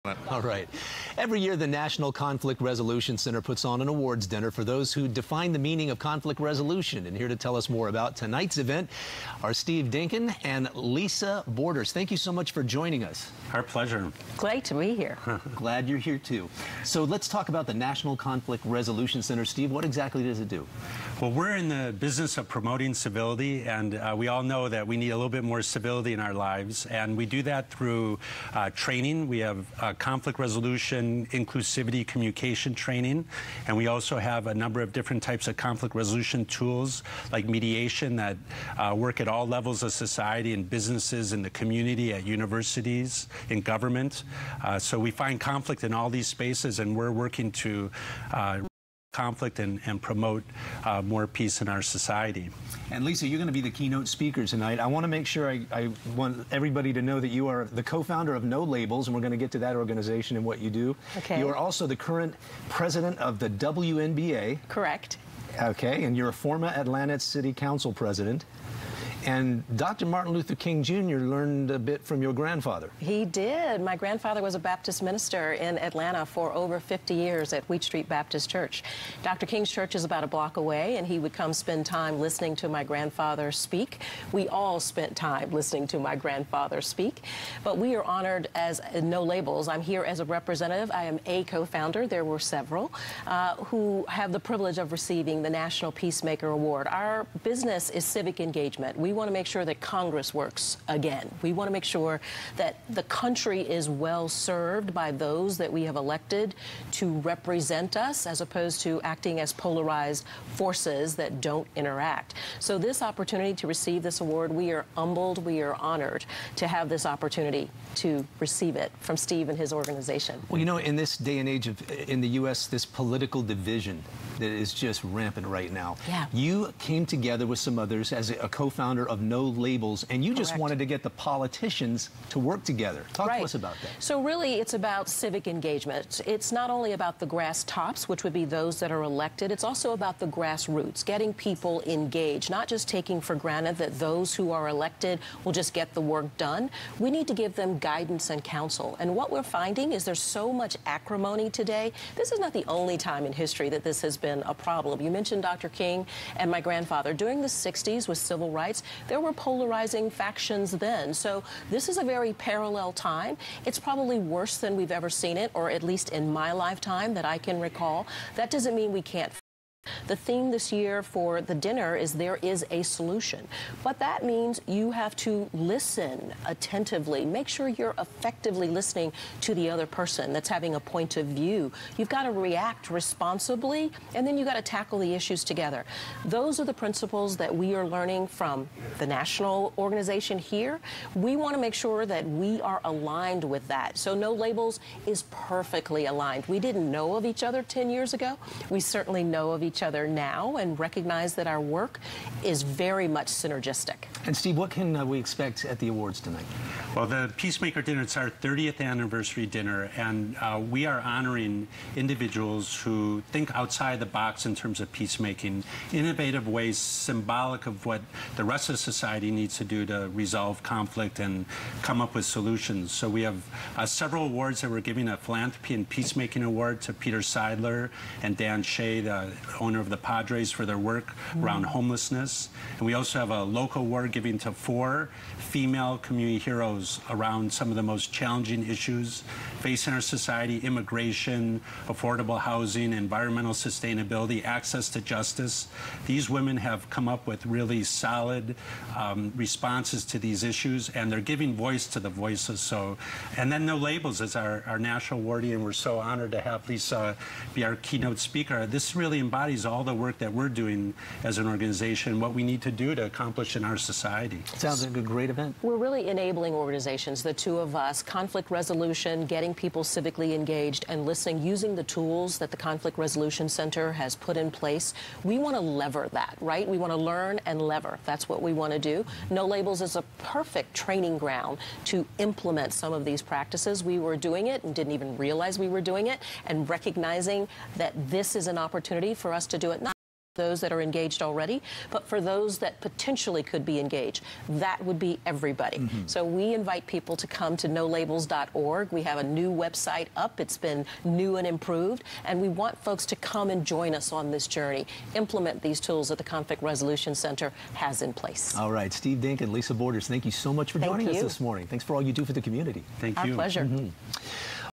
Peacemaker Awards 2018 KUSI Steve Dinkin and Lisa Borders
Steve Dinkin, President of National Conflict Resolution Center Saturday and Lisa Borders, a former Atlanta City Council president who co-founded No Labels, an organization that combats partisan dysfunction, sat down with KUSI to talk about the 30th anniversary of the National Conflict Resolution Center's Peacemaker Awards.